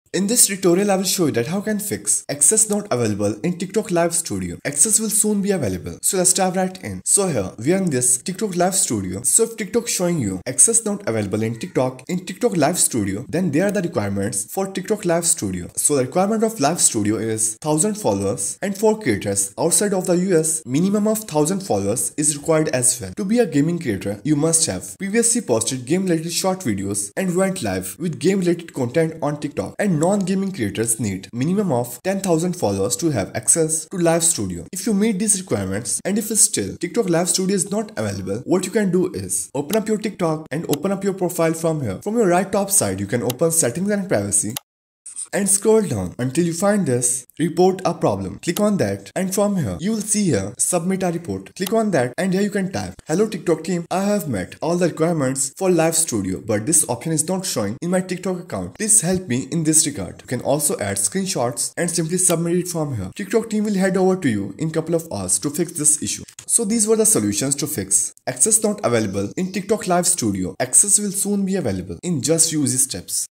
How To Fix Access Not Available In TikTok Live Studio (2025 New Method)
In this Tutorial you'll learn How To Fix Access Not Available In TikTok Live Studio in 2025.

📱🚫 Title: How To Fix Access Not Available In TikTok Live Studio

📌 Video Description:

Unable to access TikTok Live Studio? This tutorial will guide you through the steps to resolve the "Access Not Available" issue and get you live streaming smoothly.

We'll cover:

Ensuring your account meets the requirements for TikTok Live Studio.
Verifying your account and settings.
Updating the TikTok app.
Contacting TikTok support if necessary.
🔧📺 Stream Without Issues!
Watch the video to learn how to fix access issues in TikTok Live Studio and start streaming effortlessly.

By The End Of This Tutorial, You'll Have a Fully Known By How To Fix Access Not Available In TikTok Live Studio